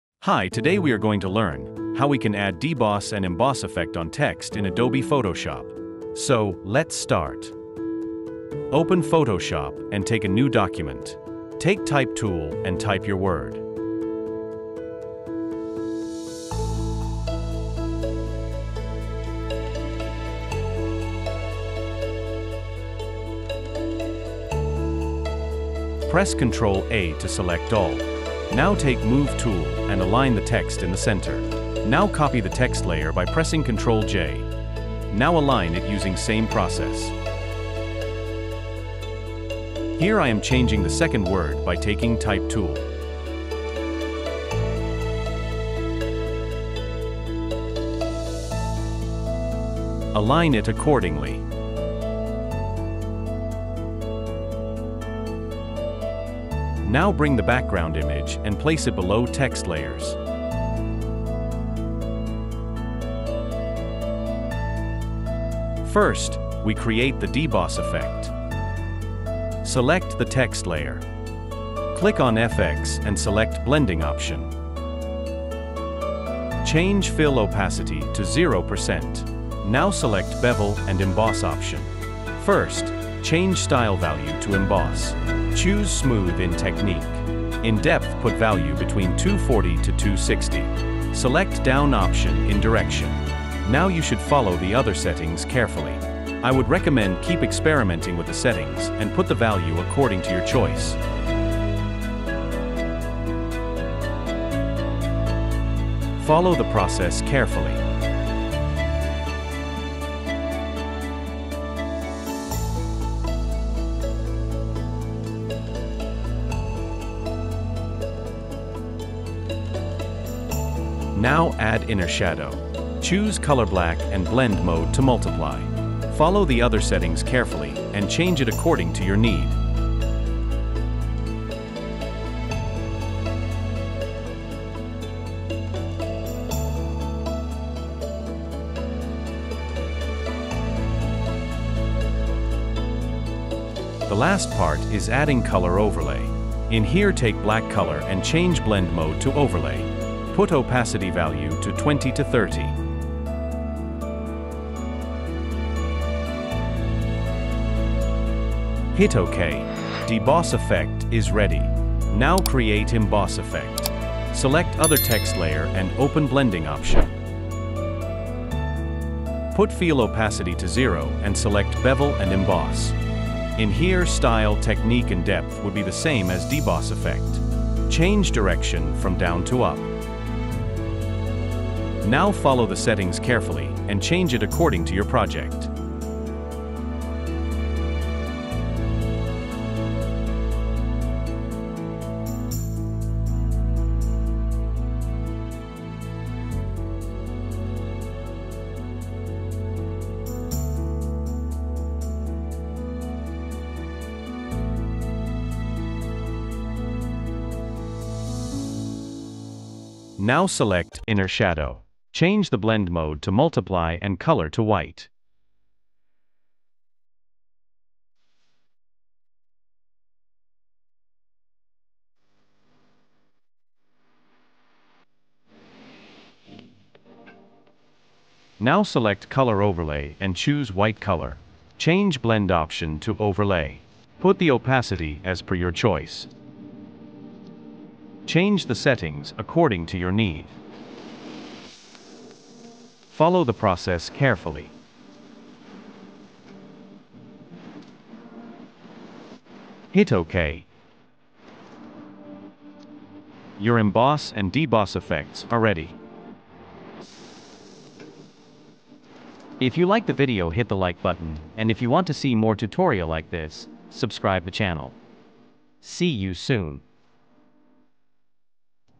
Photoshop text effects | Letterpress Effect | Photoshop tutorials
Hello , welcome to Tuts Gallery. today we are going to see how we can create Letterpress effect in Photoshop.